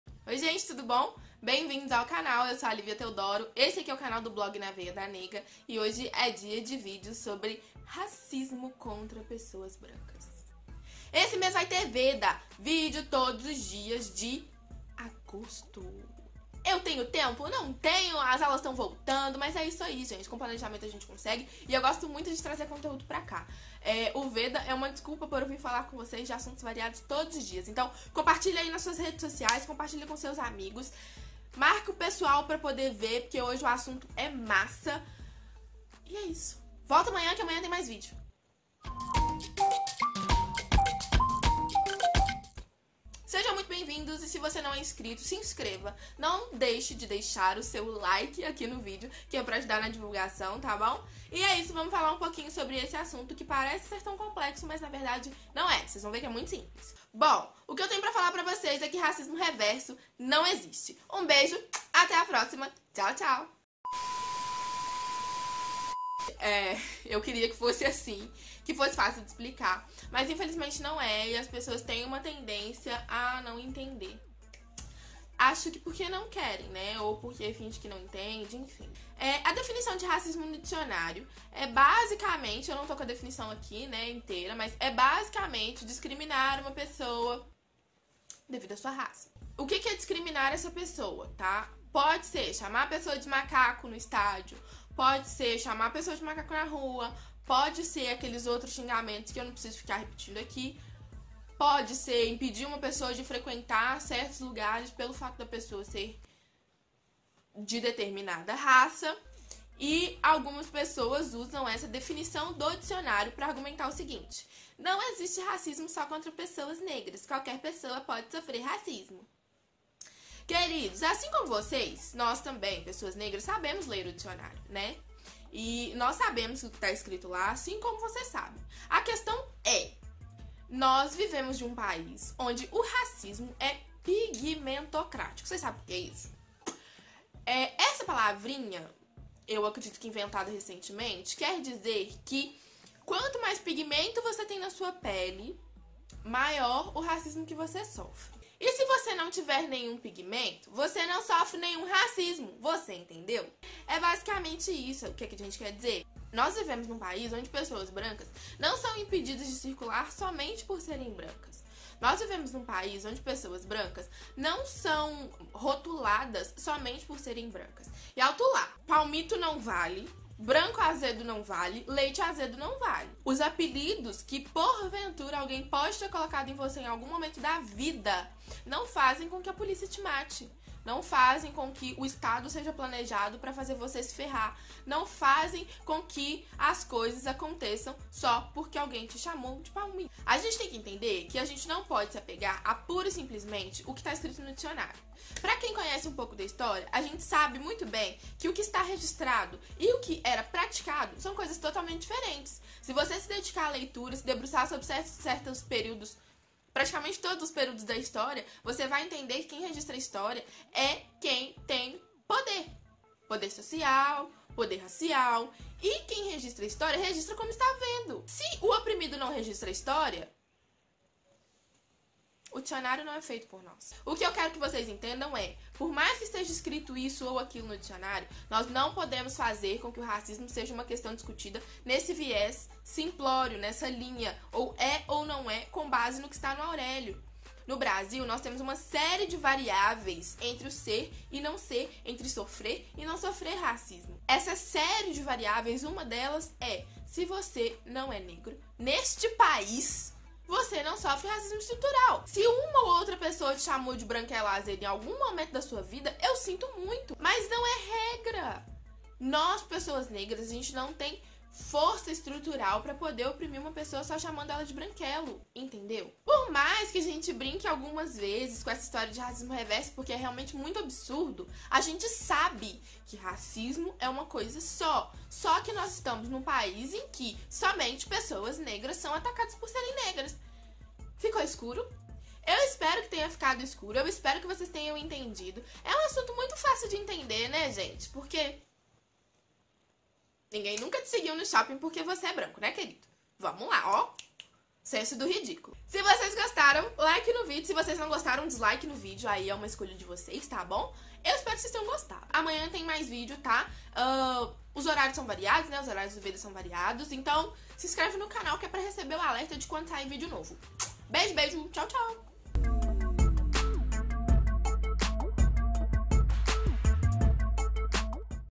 OS CASOS DE RACISMO CONTRA PESSOAS BRANCAS [VEDA PRETA 02]
A proposta de hoje é discutir o que muita gente chama de "racismo reverso", que na verdade, não existe. Racismo é possível de acontecer com qualquer raça, segundo o dicionário.